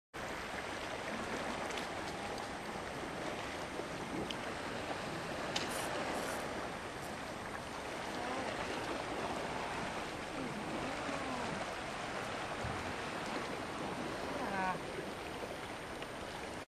小魚の群れを狙うダツ ５
和歌山の釣太郎です。田辺元島（もとしま）堤防で。